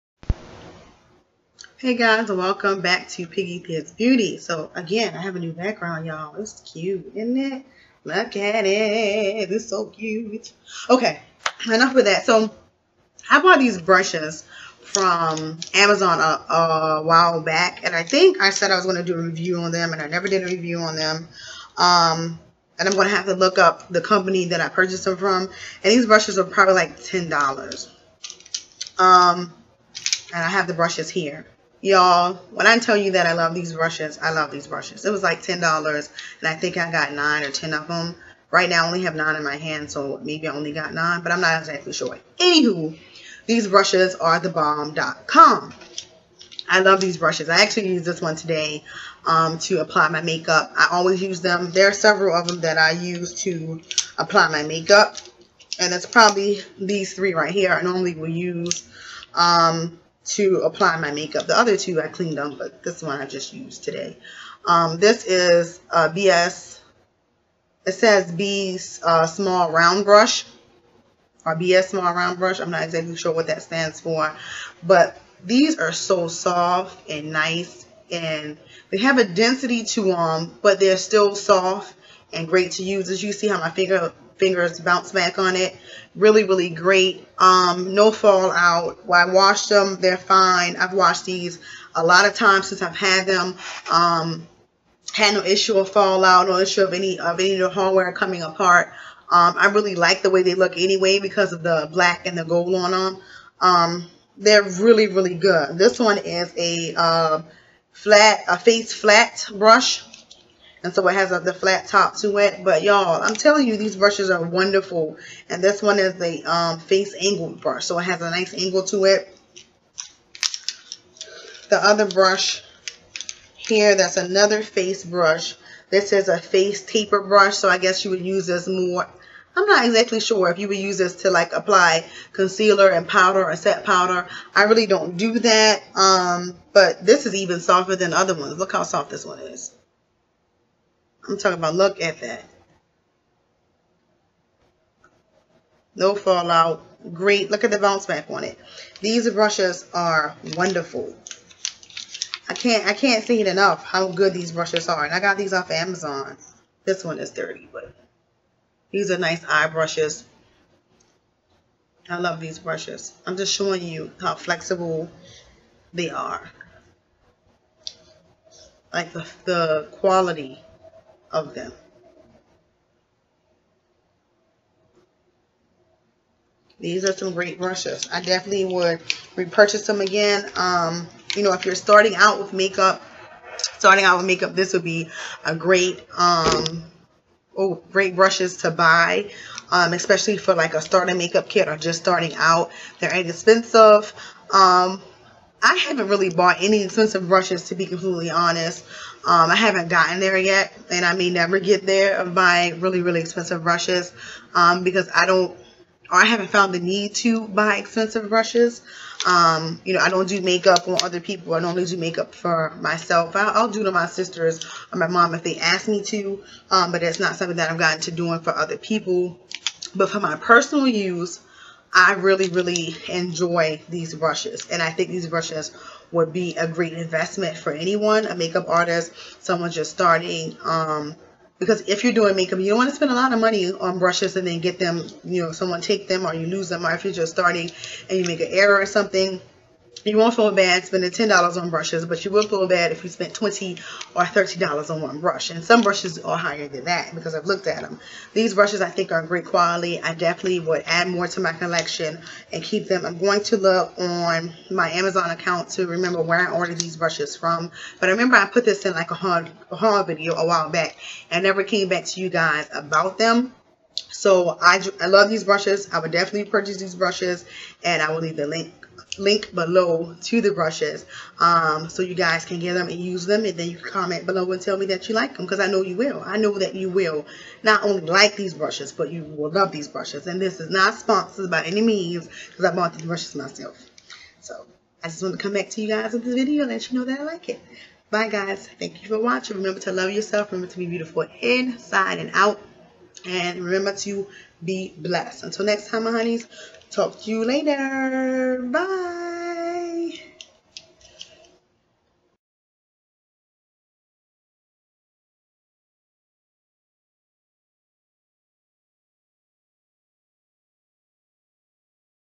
Brush review on BS-Mall brushes
There are 10 brushes n this set